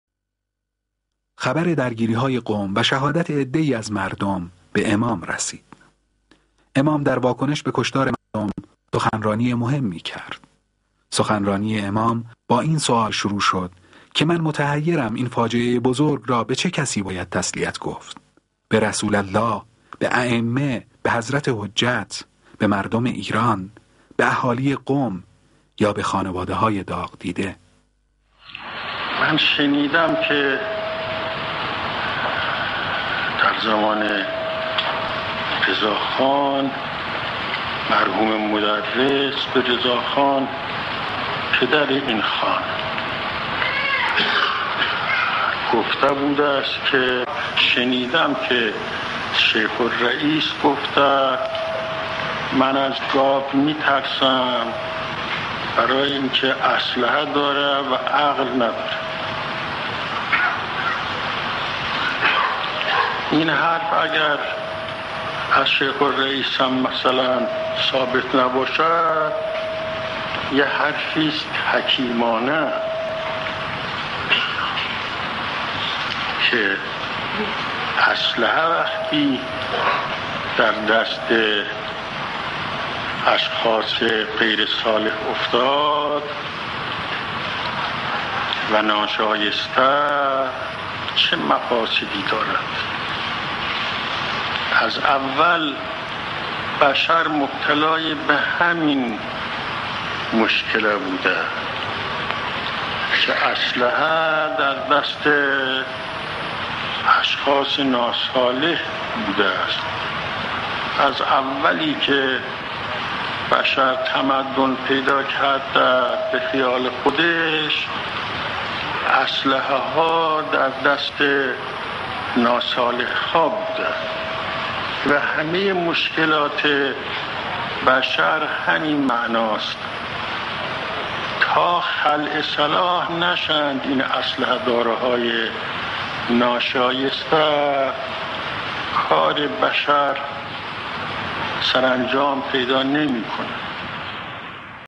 [English Translated] Imam Khomeini Speech in Najaf - Masjid Sheikh Ansari - Persian.flv
Imam Khomeini Speech in Najaf - Masjid Sheikh Ansari